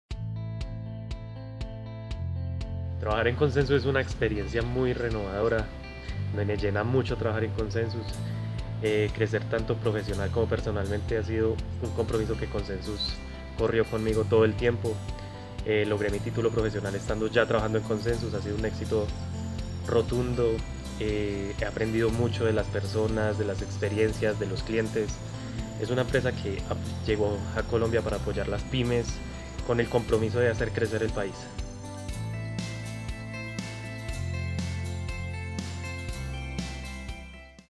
AlmadeMarcaConsensus - Cristian Gómez, Analista de Soporte
"Ha sido un éxito rotundo aprender de las personas, de las experiencias, de los clientes" Cristian Gómez, Analista de Soporte